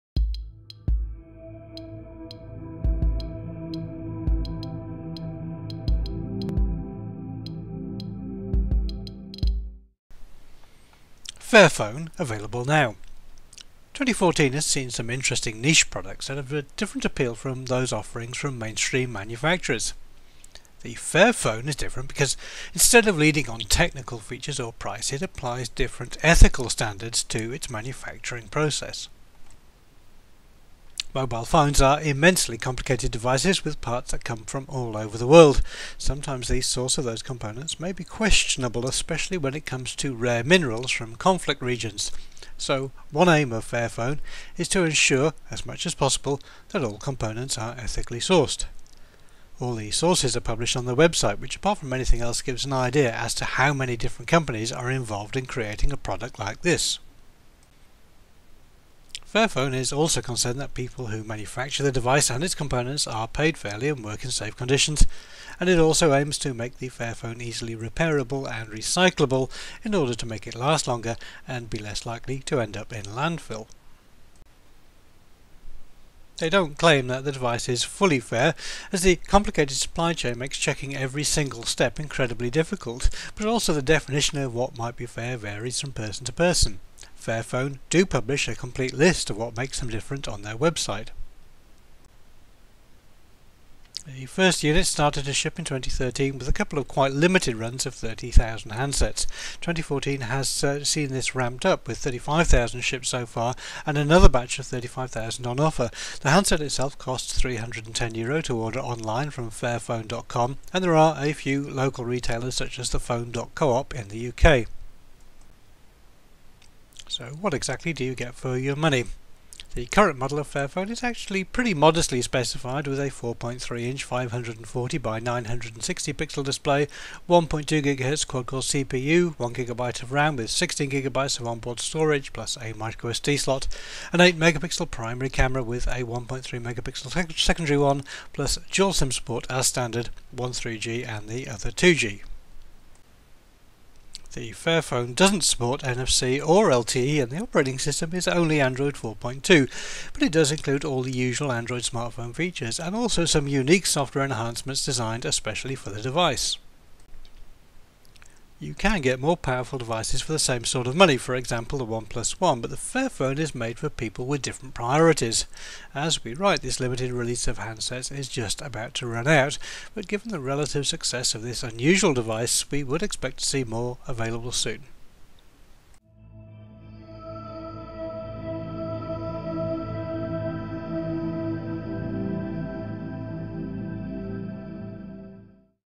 Fairphone
The Fairphone differentiates itself from the competition paying greater attention to where all the components comes from to ensure that they are ethically sourced.